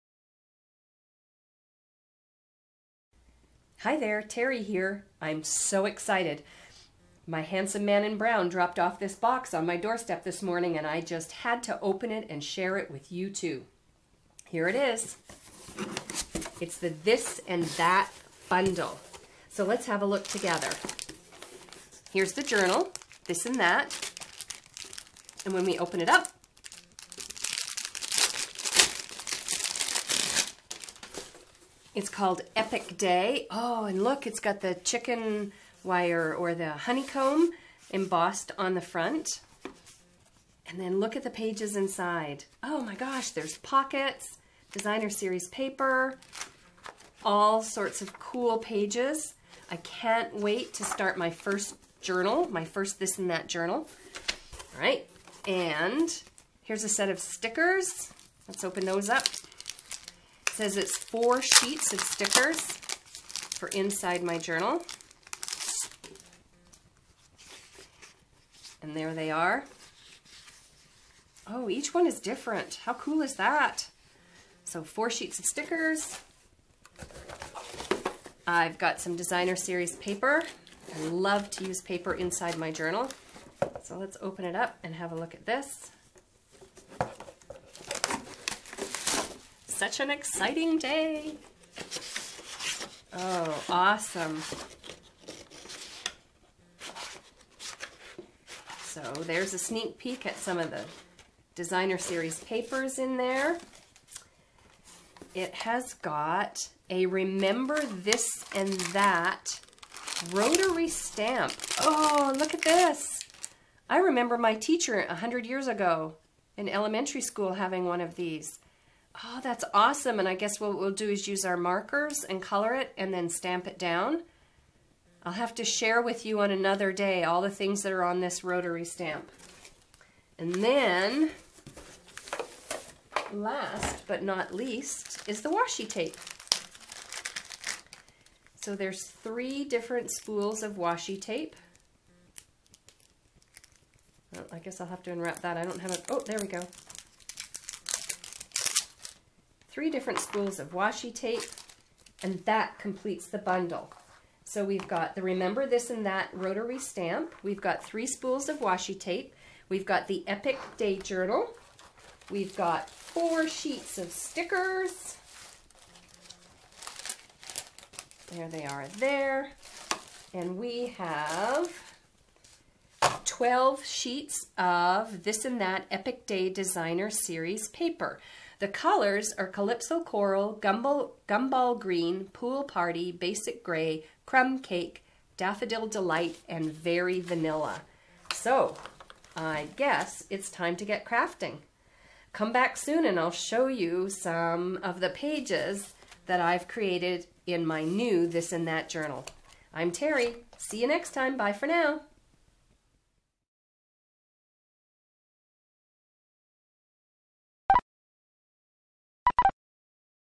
This and That Bundle
Introducing the This and That Bundle featuring the Epic Day journal and other supplies for keeping your precious memories in one super cool spot!

Inky hugs,
Terri